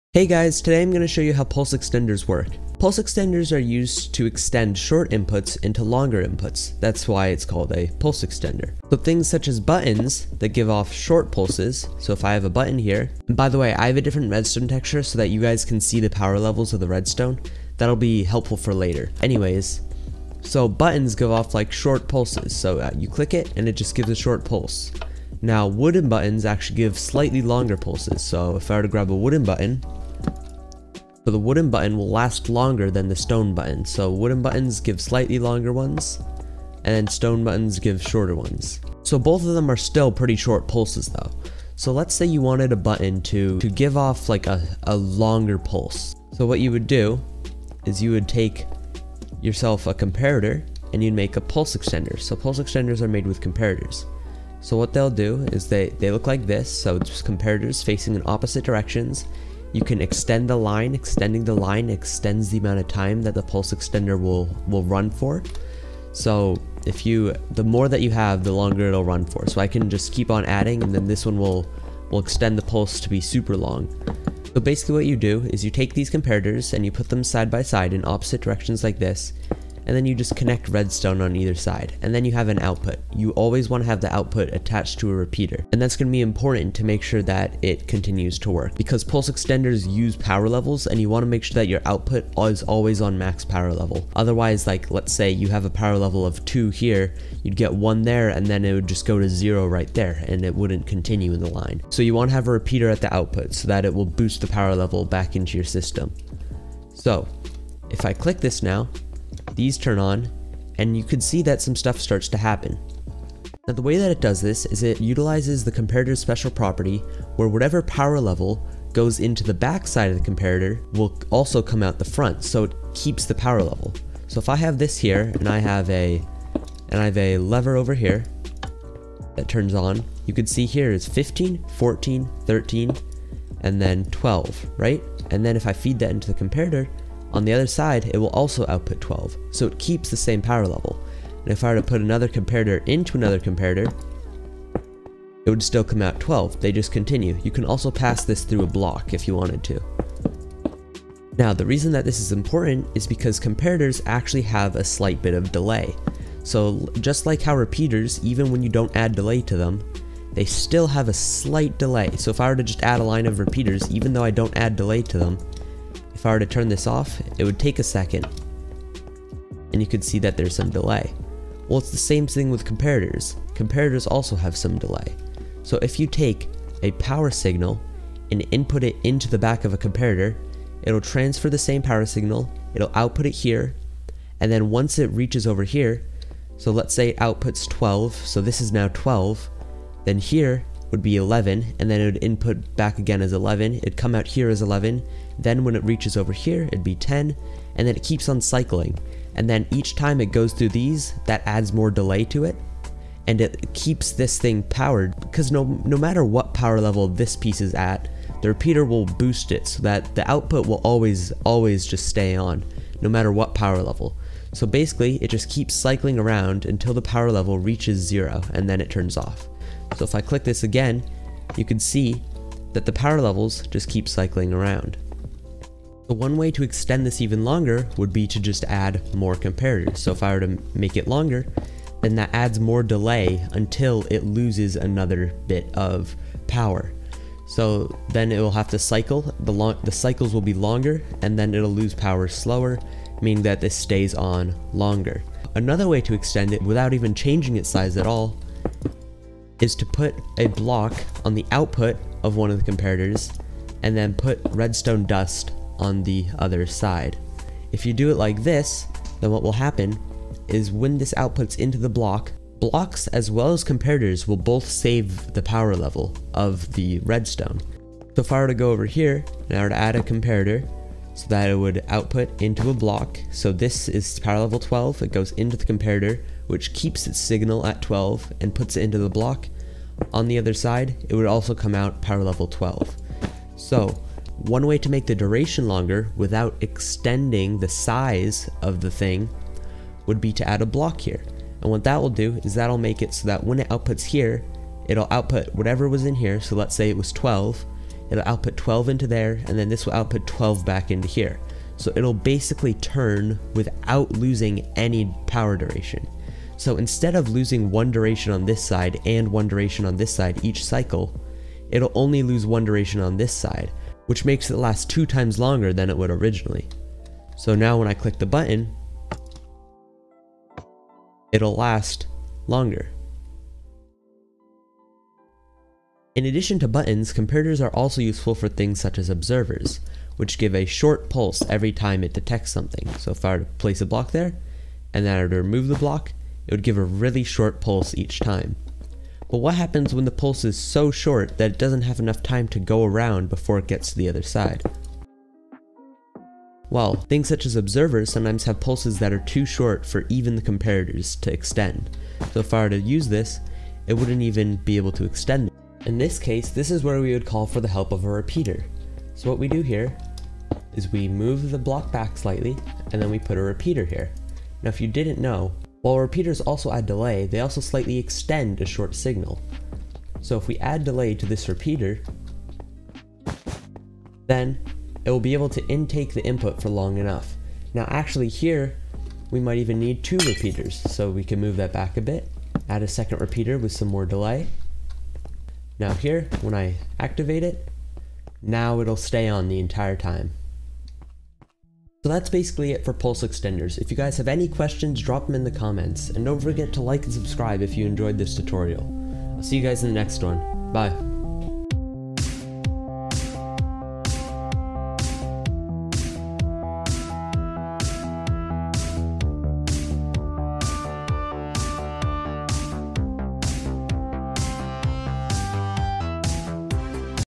How do Pulse Extenders Work?
In this video I teach you how to make pulse extenders and how to modify them to be the exact length that you want them to be.